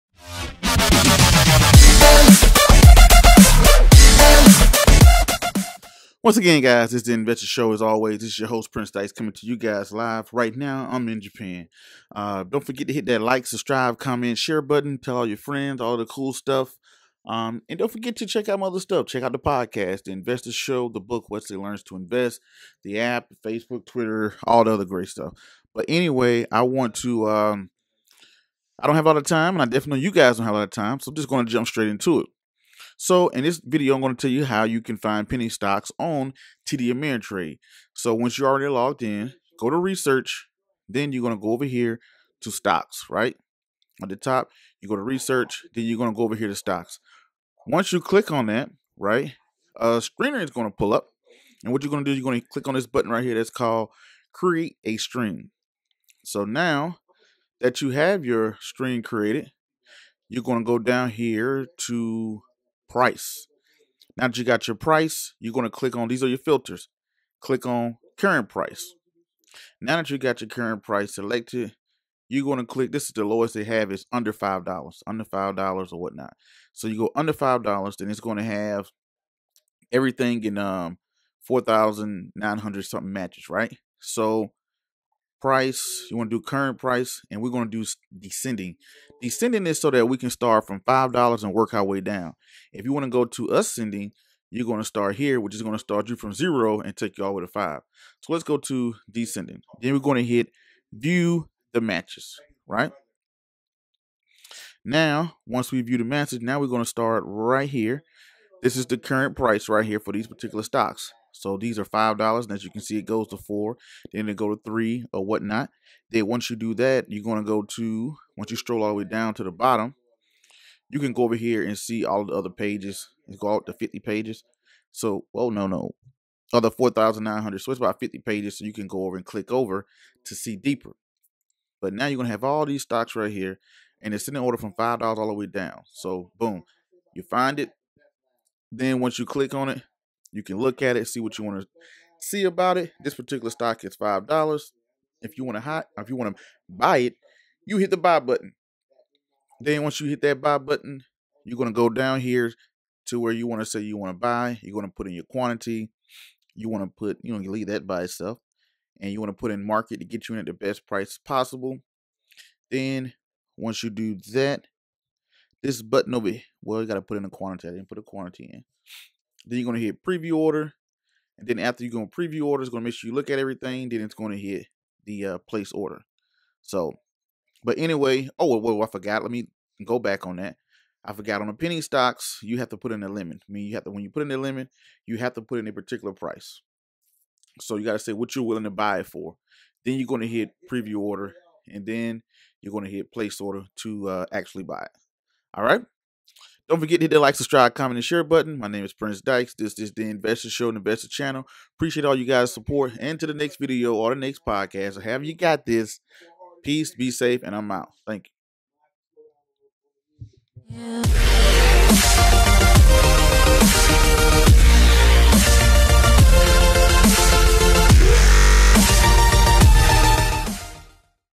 How to find and buy penny stock w/ TD Ameritrade (3 min)
Step by step of how to find and buy penny stocks with td TD ameritrade